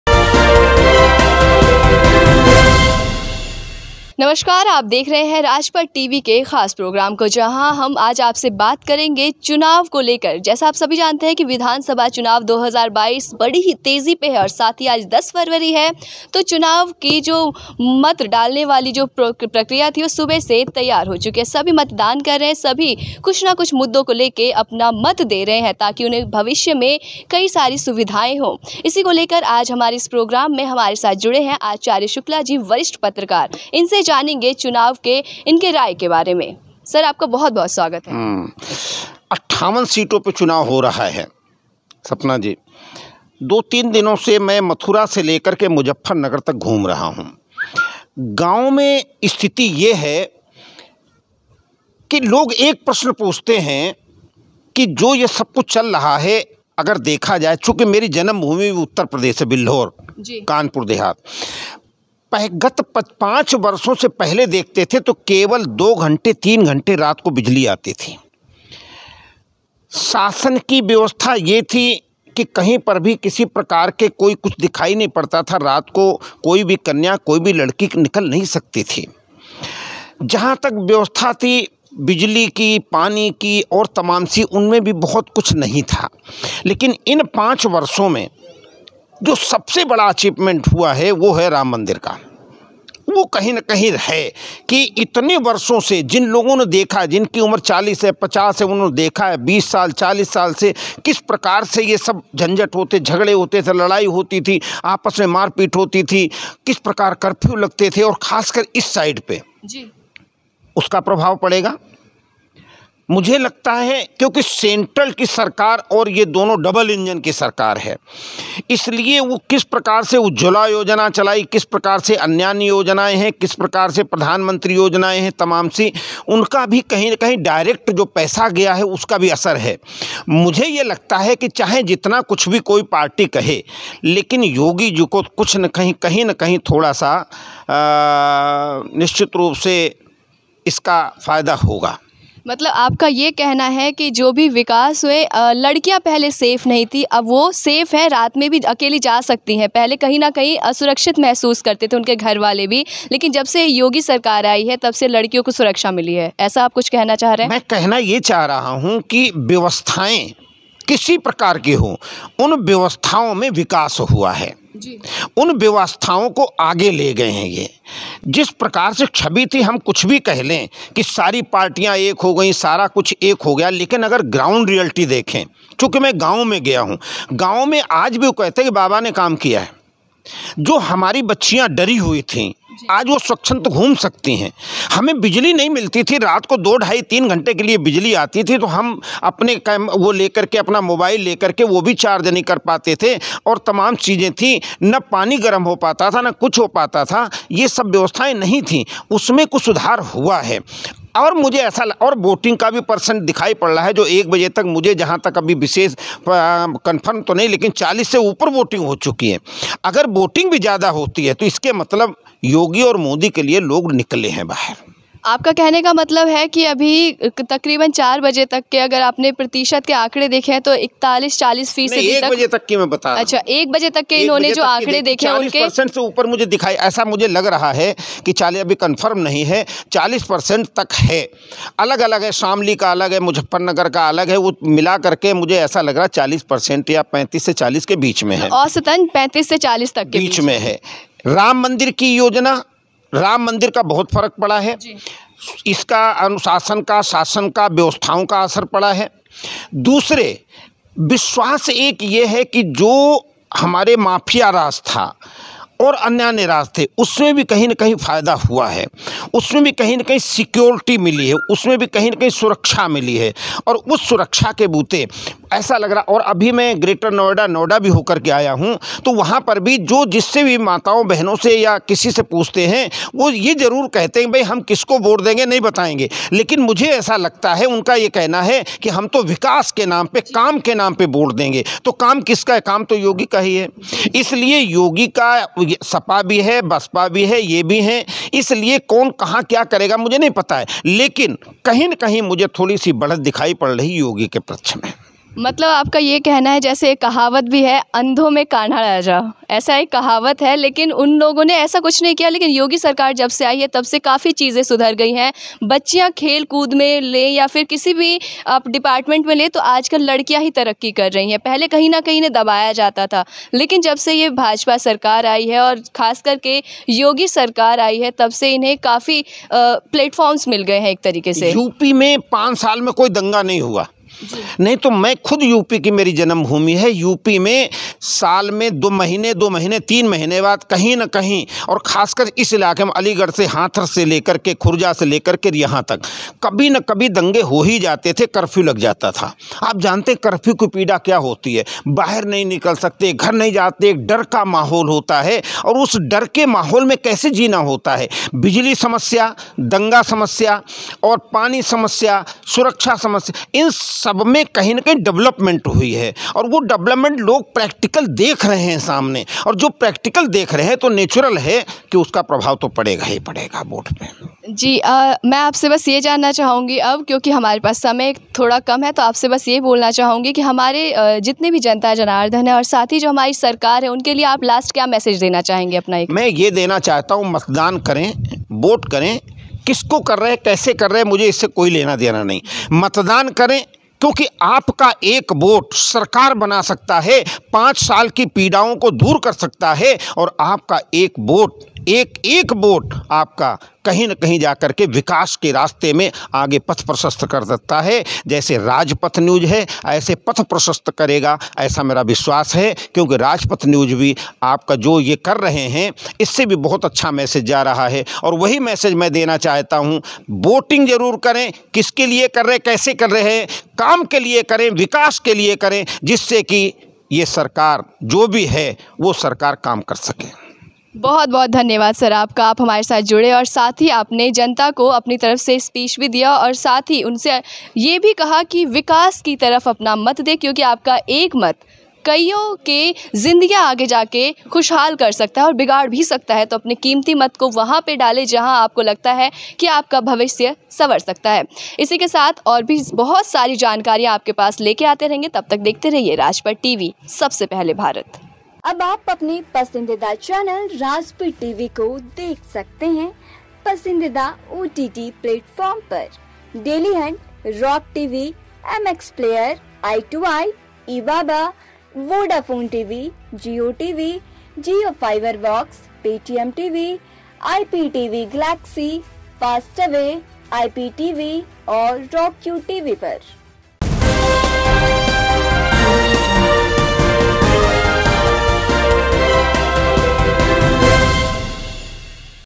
आचार्य शुक्ला जी से खास बातचीत... Rajpath TV
यूपी में पहले चरण का मतदान
कौन बनेगा 2022 का बादशाह?
आचार्य शुक्ला जी से खास बातचीत
पहले दंगे होते थे अब नहीं होते’
चुनाव में किसका पलड़ा भारी?
जानिए वरिष्ठ पत्रकार आचार्य शुक्ला जी से.